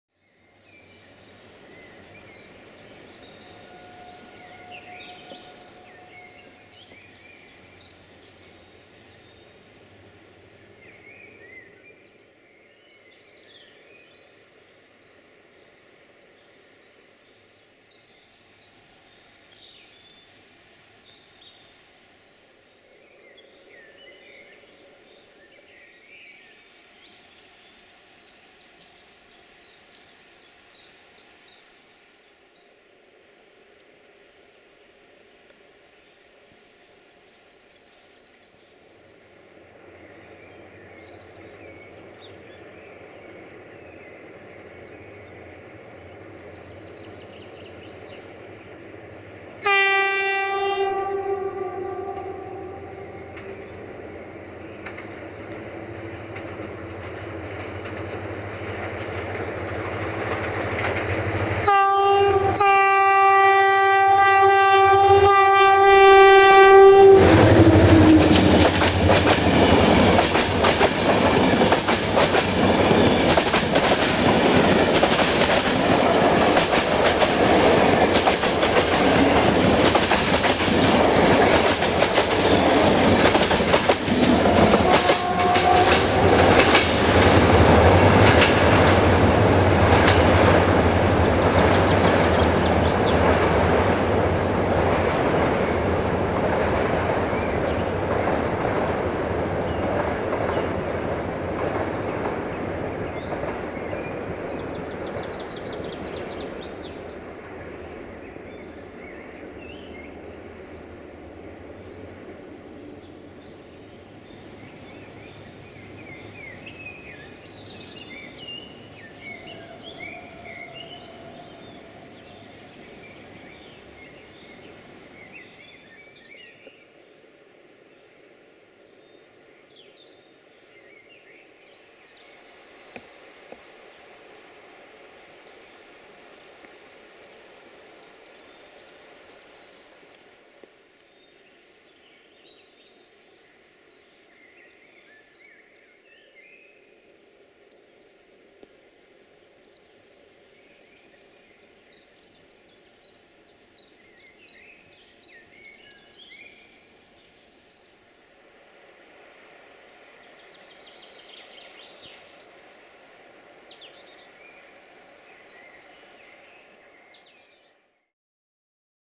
GM 64-1138-8 & IR1742 Satu Mare-Bucuresti Nord in Defileul Crisului Repede`s Canyon - 09 April 2017
1:IR1742 - Satu Mare - Bucuresti Nord - Jimmy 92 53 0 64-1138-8 RO-SNTFC

09 April 2017